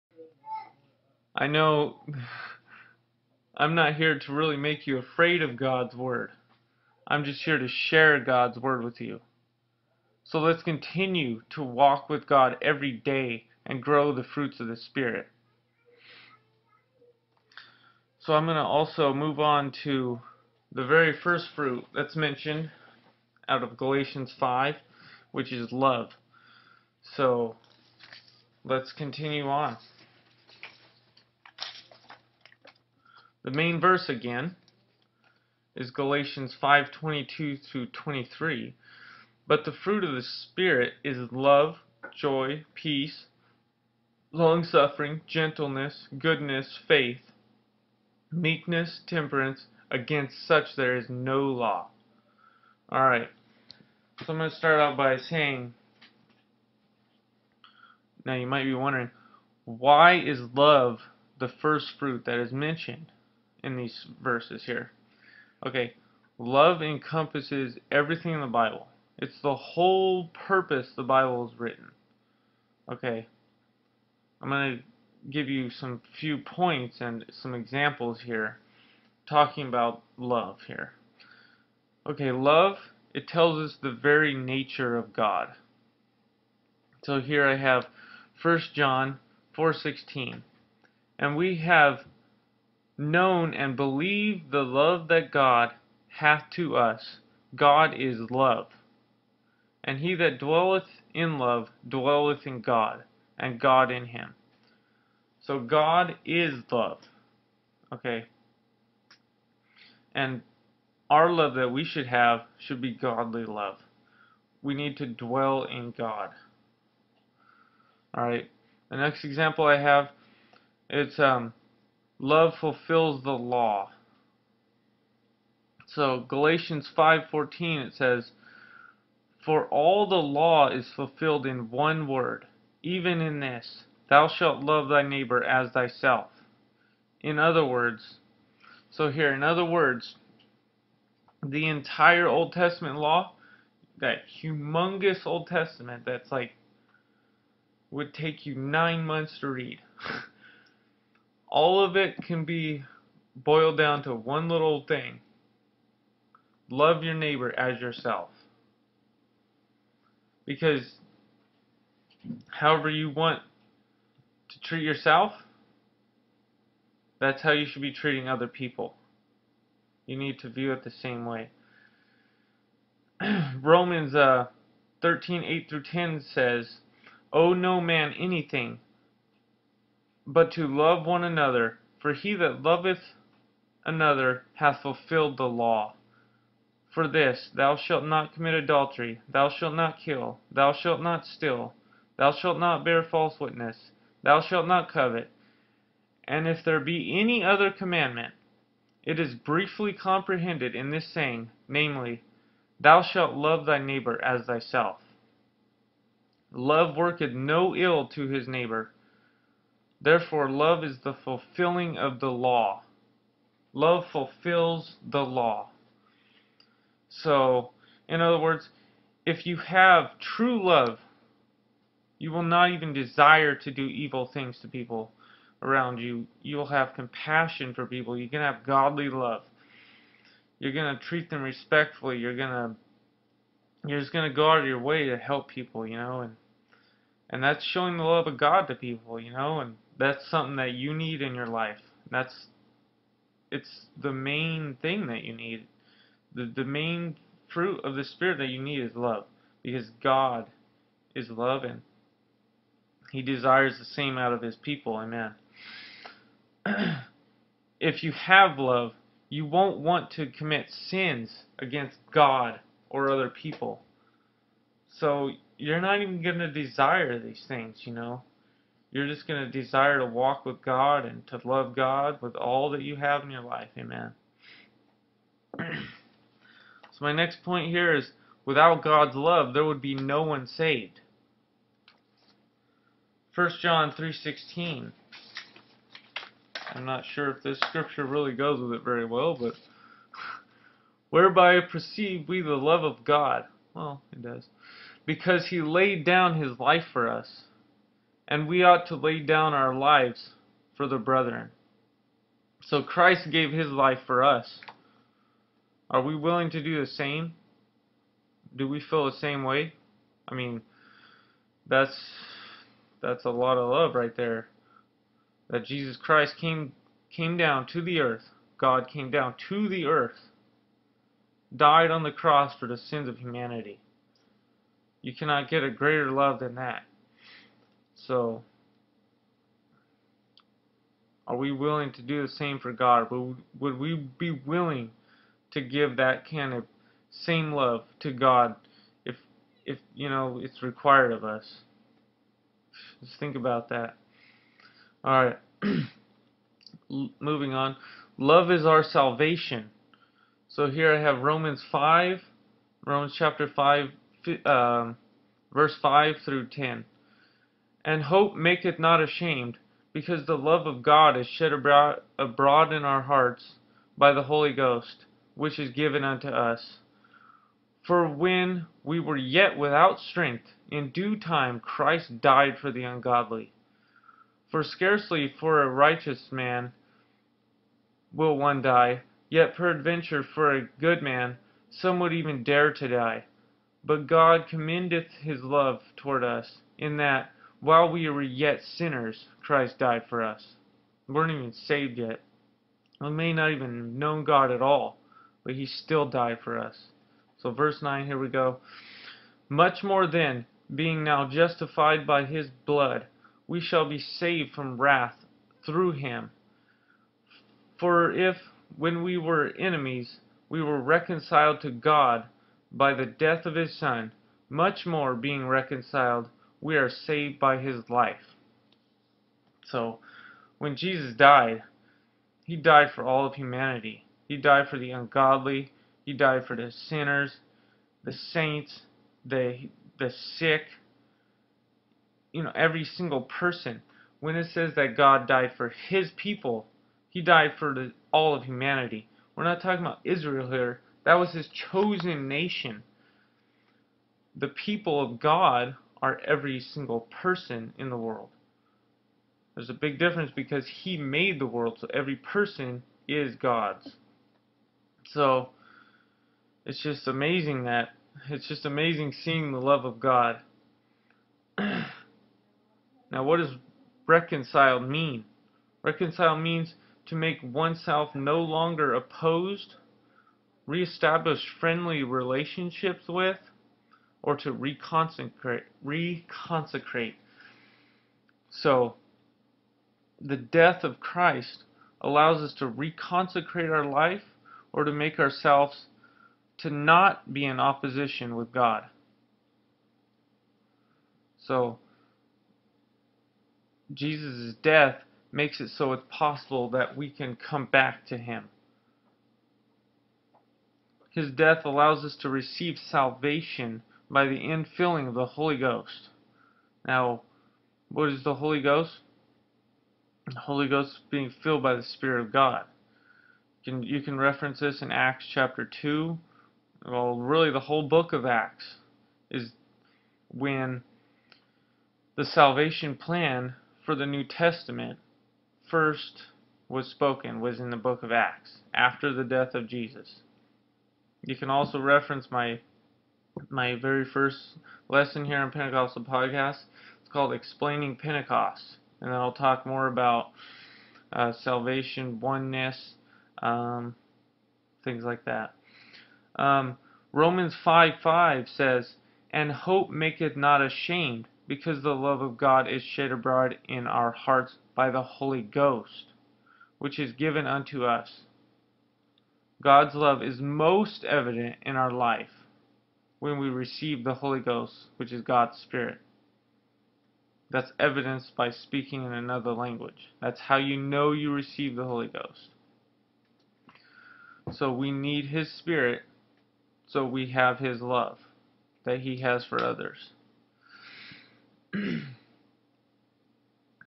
Fruits of the Spirit Love Part 2
Lesson on 1-26-13 about the 1st fruit of the Spirit (Love)
search for a local pentecostal church @ upci.org